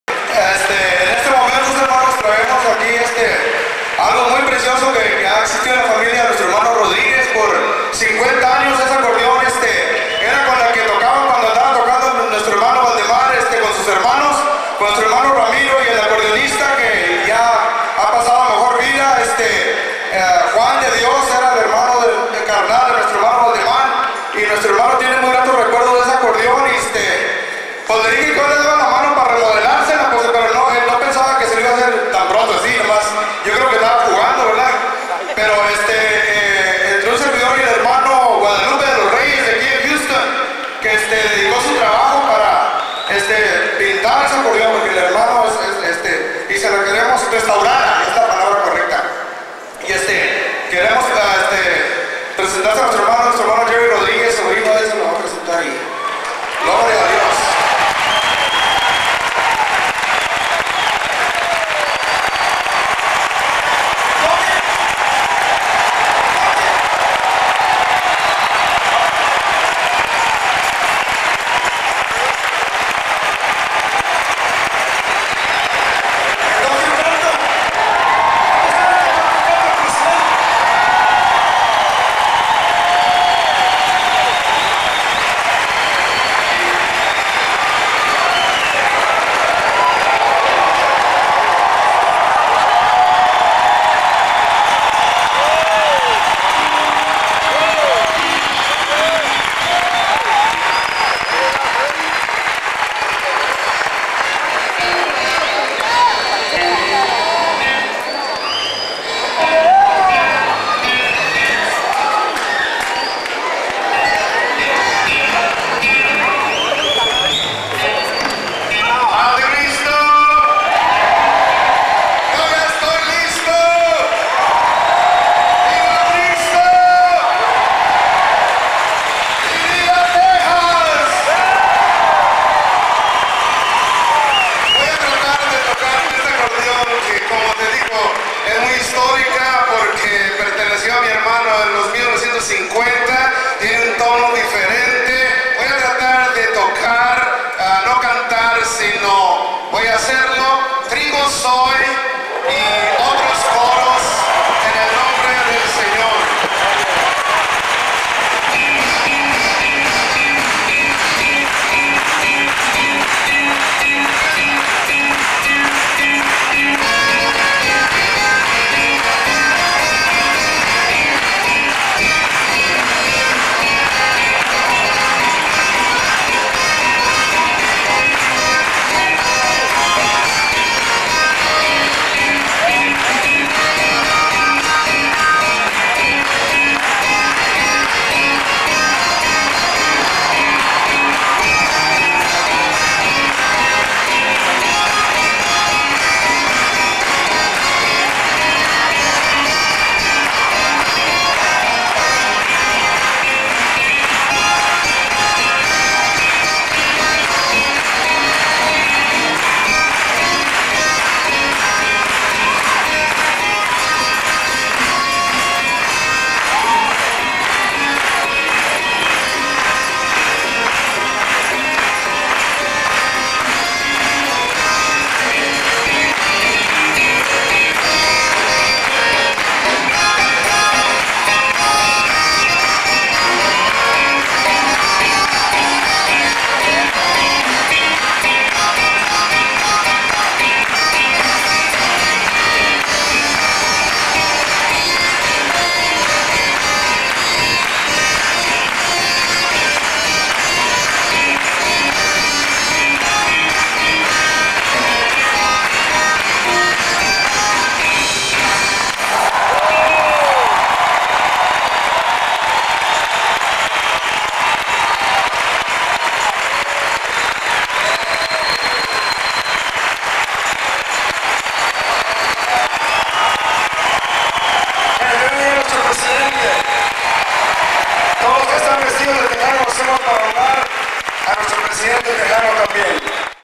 Baldemar Rodriguez - Dedicación del acordeón histórico
Dedicación del acordeón histórico durante la
Convención Nacional en Houston, Texas 2001.
Acordeón - Obispo Presidente Baldemar Rodríguez
Bajo Sexto - Jacinto 'JP' Prado
Bajo - Raúl Hernández
Batería - Gamaliel Hernández
Presentación - Elías Cabrera